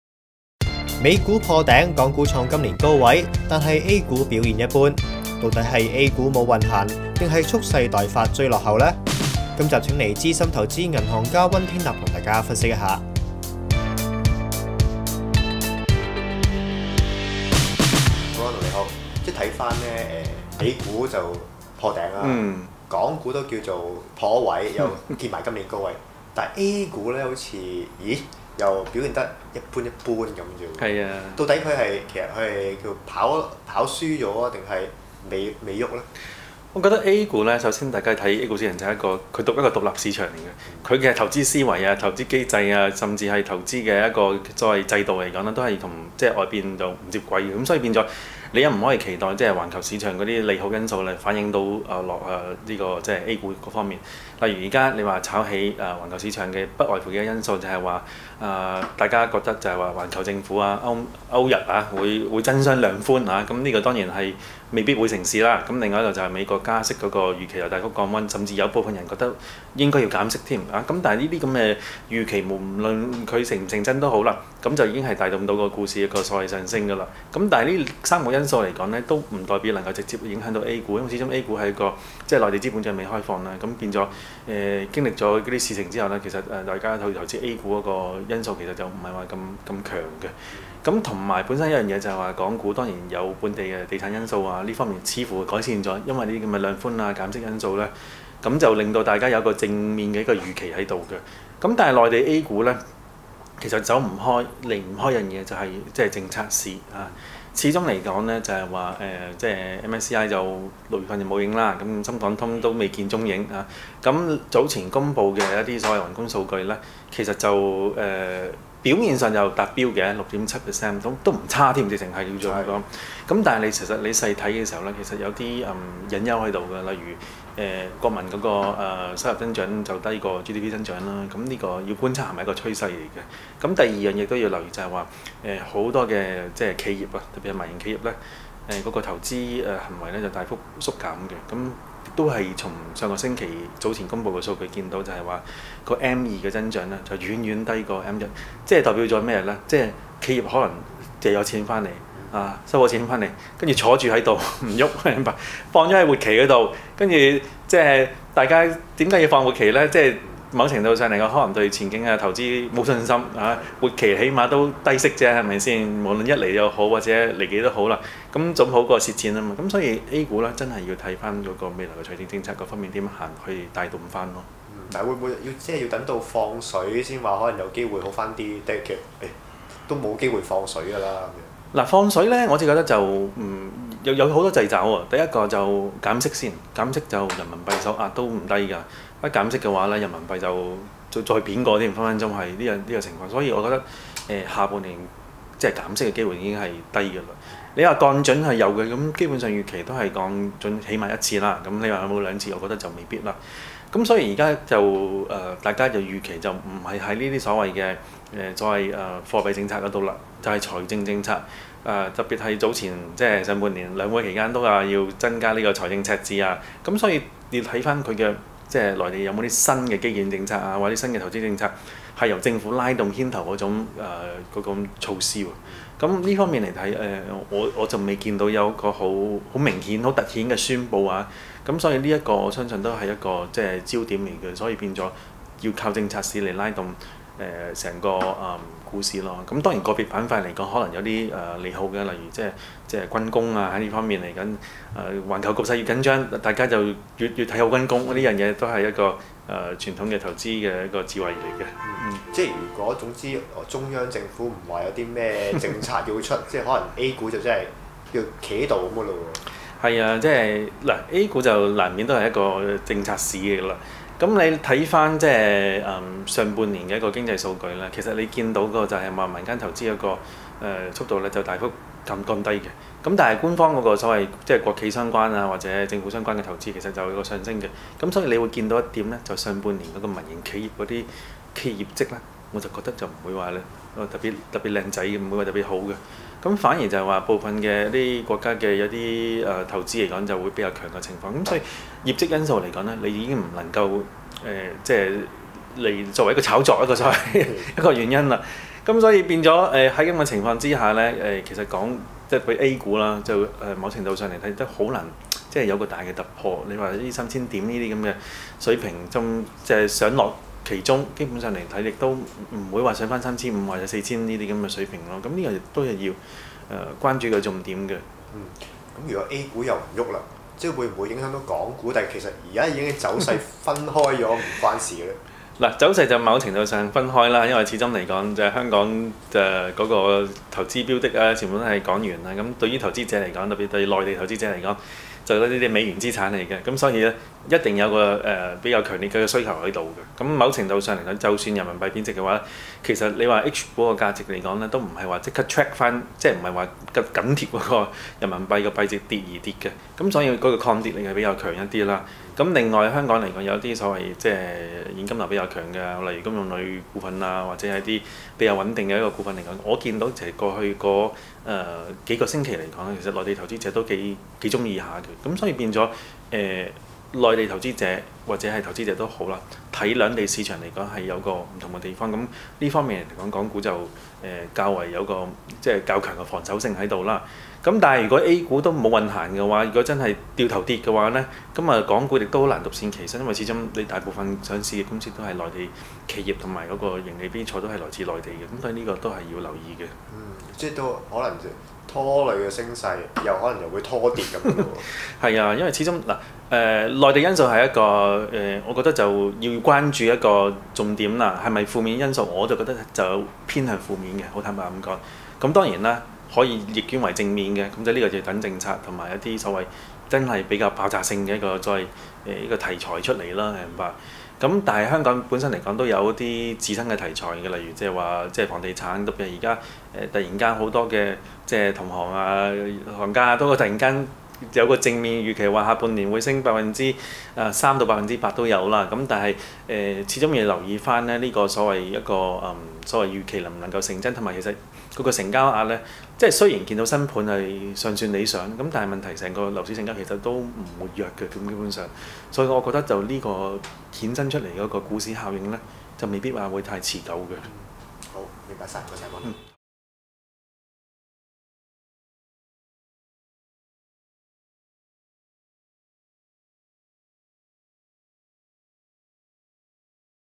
中港股市背馳 溫天納拆局（2016年7月22日）
【香港經濟日報】美股破頂，港股創今年高位，惟A股表現一般。到底A股是無運行，還是蓄勢待發追落後？本集請來資深投資銀行家溫天納為大家分析。……（更多內容：